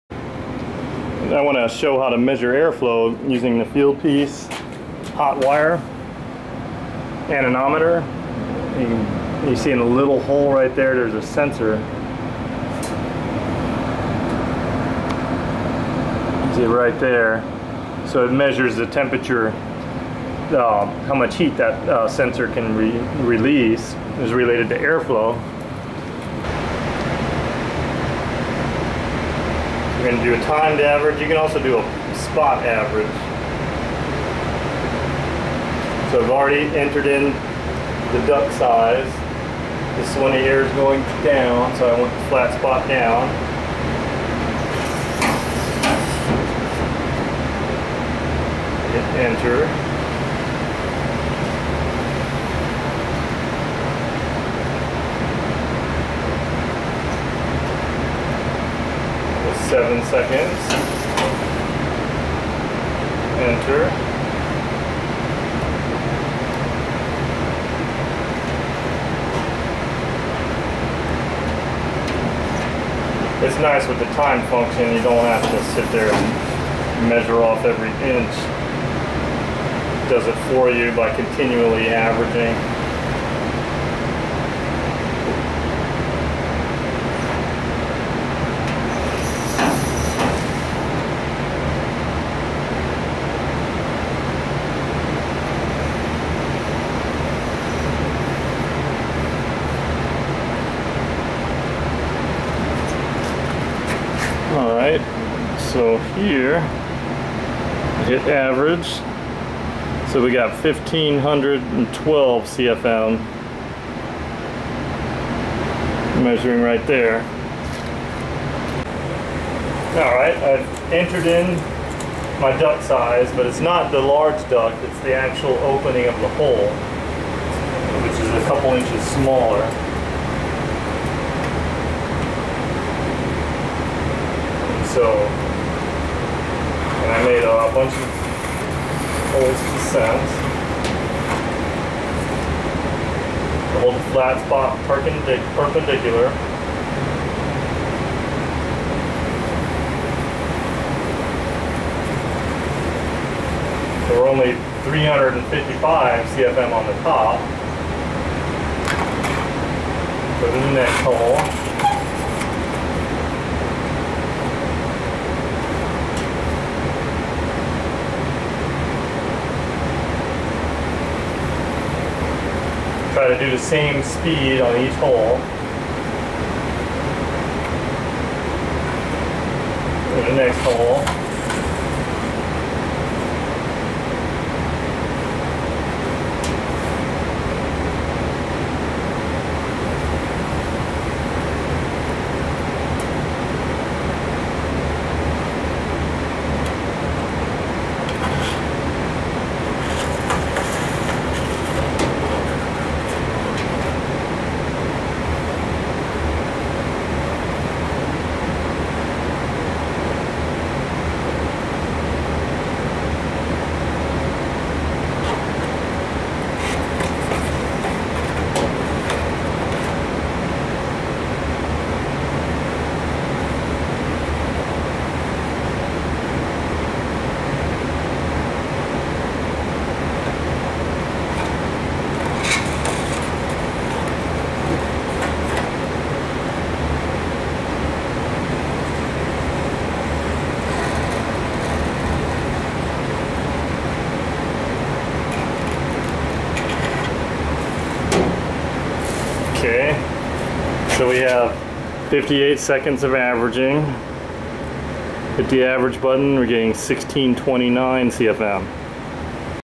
Duct Airflow with heated wire probe
The Fieldpiece STA2 is very accurate for measuring air flow inside a duct.